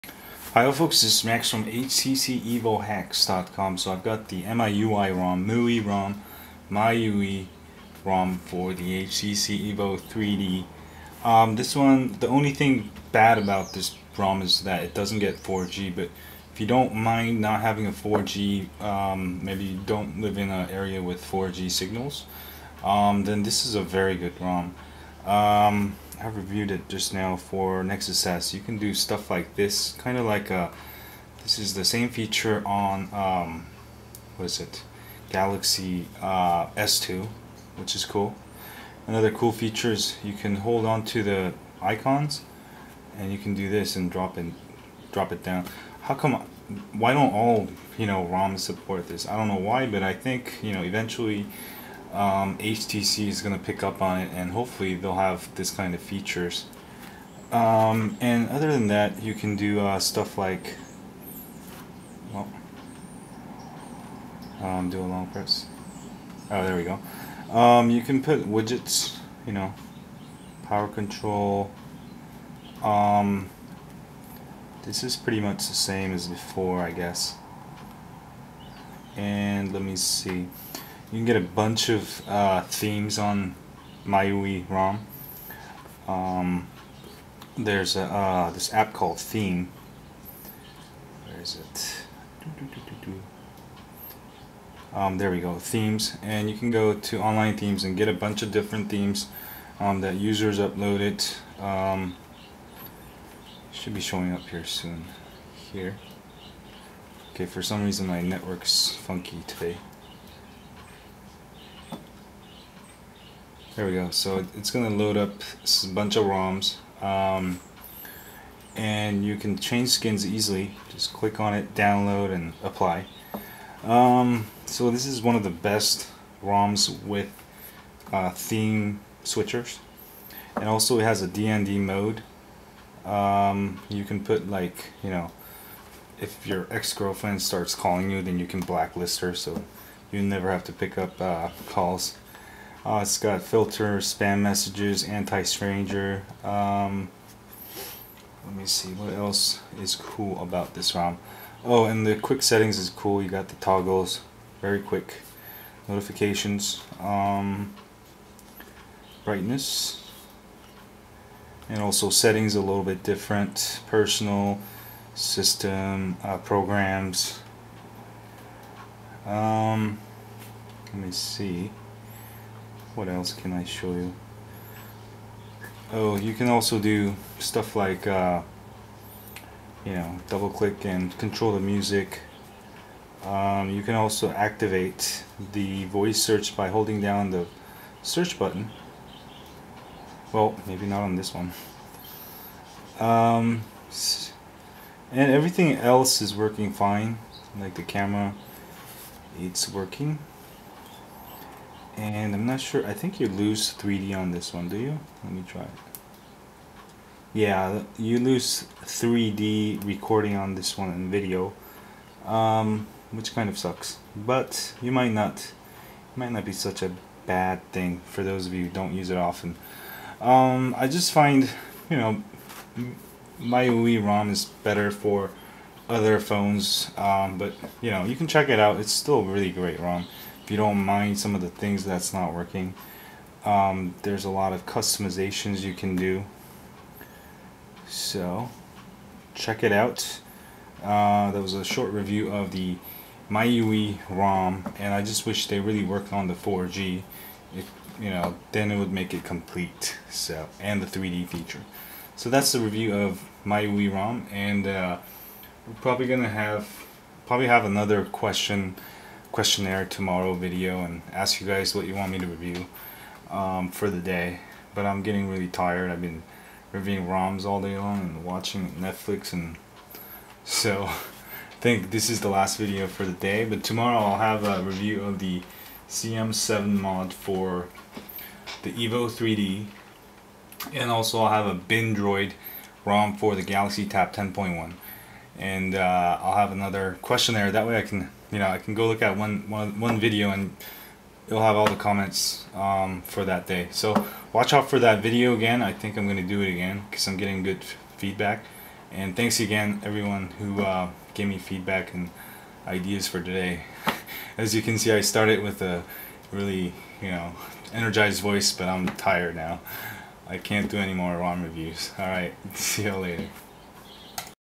MIUI ROM for Rooted HTC Evo 3D!
Here's MIUI ROM for rooted HTC Evo 3D!

Want to stay updated on latest HTC Evo 3D hacks and tips?

Sign up for our HTC Evo 3D Hacks Newsletter here:

HTC Evo 3D reference

For all ROMs, you need to root your Evo 3D!